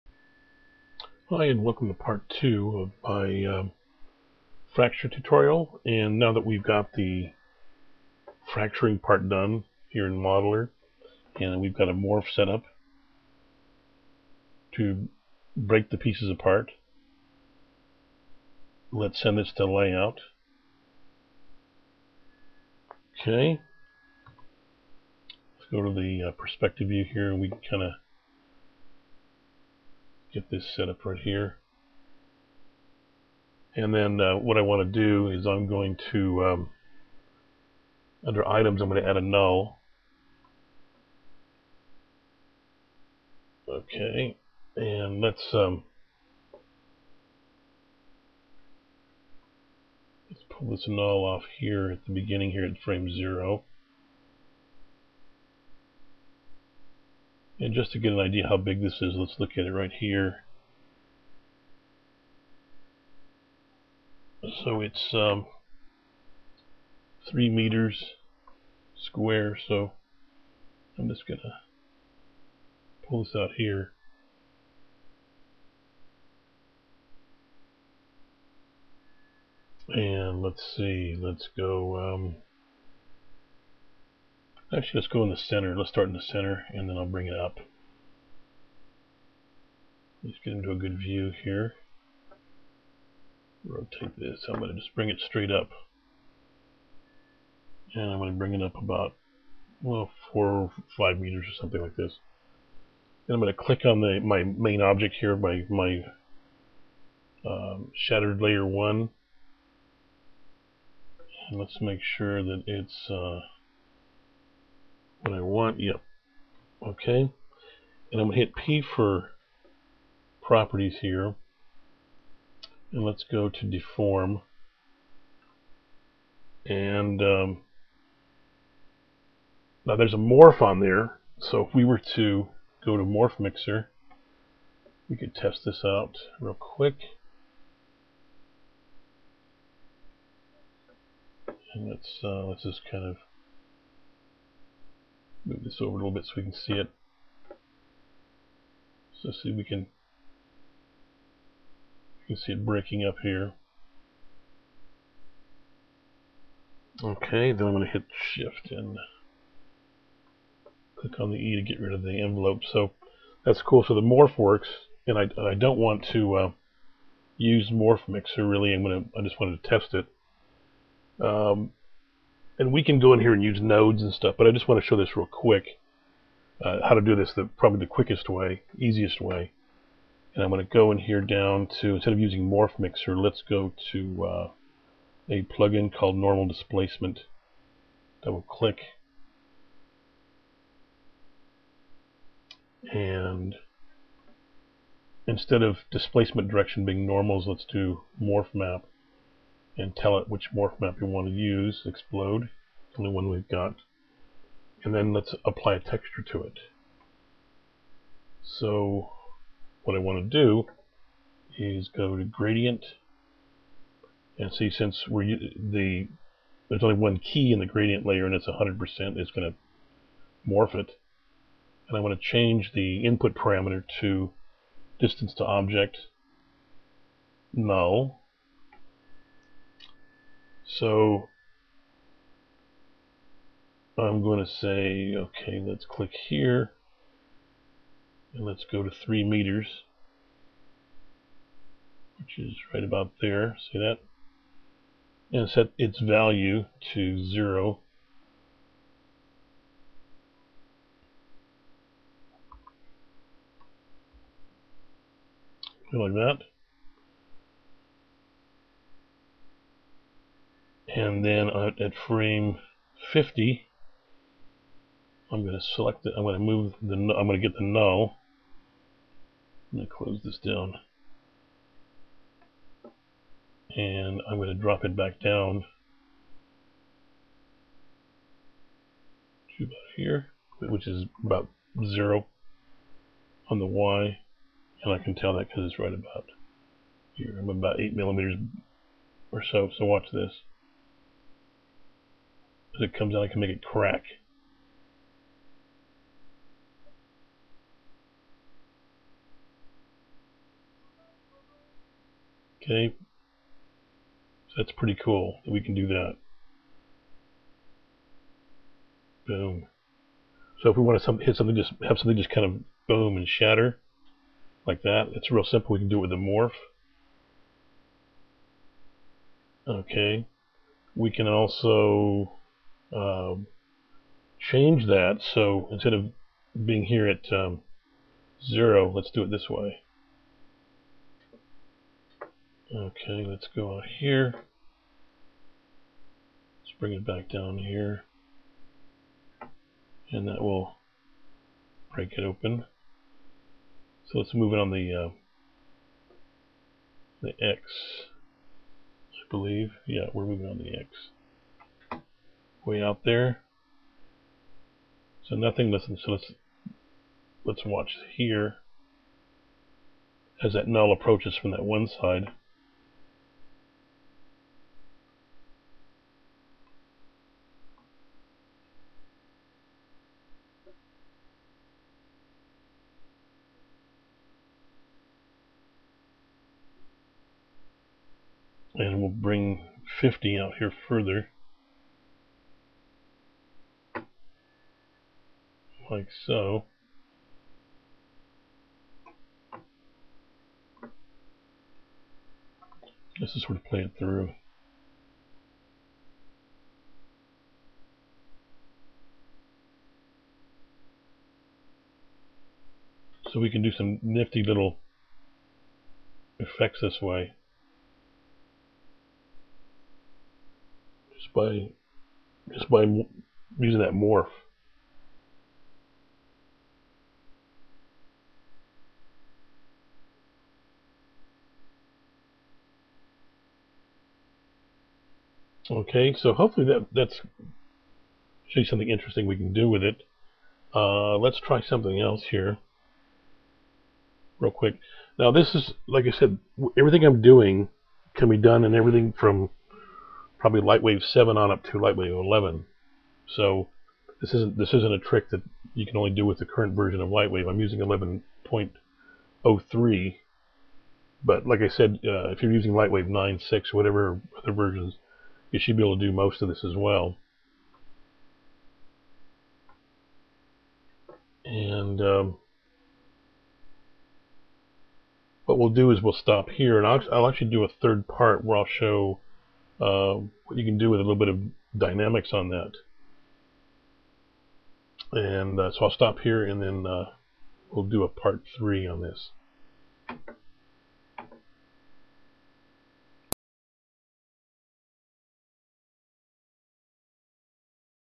TUTORIAL: Part2 - Fracturing using an Endomorph
Part 1 was fracturing the geometry without the fracture plugin. Part 2 is how to animate the fracturing using an endomorph and a gradient texture.